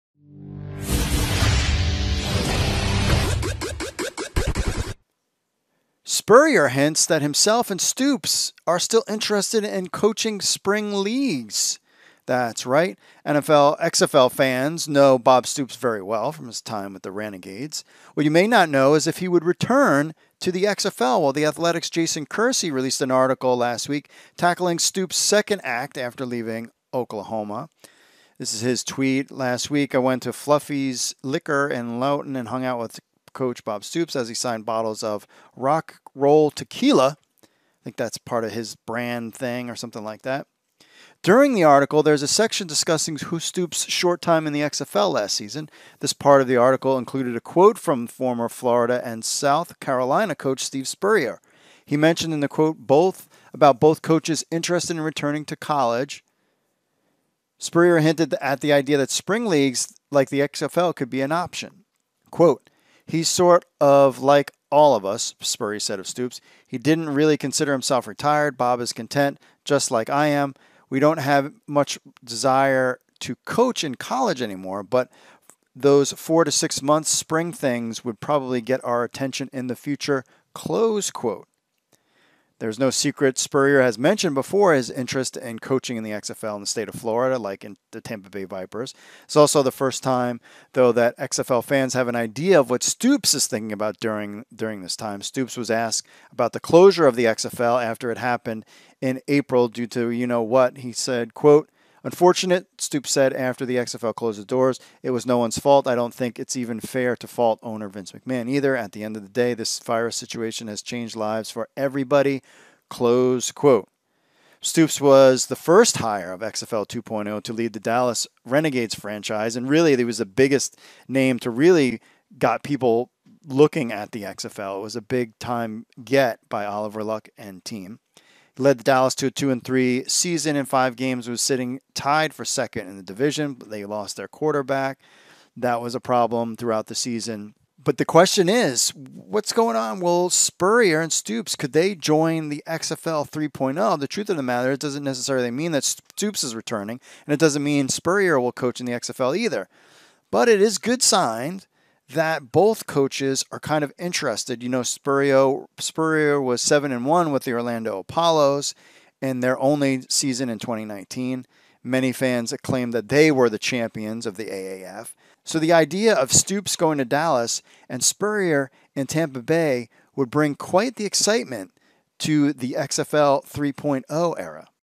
Spurrier Hints Himself, Stoops Interested In Spring Leagues Like XFL [XFL News]
XFL fans would be excited to know there’s interest from two legendary college coaches: Steve Spurrier and Bob Stoops.

Now XFL fans know Bob Stoops very well from his time coaching the Dallas Renegades last season. What many don’t know is if he would return to the XFL next season or not.

The Athletic’s Jason Kersey released his article Thursday tackling Stoops’ second act after leaving Oklahoma.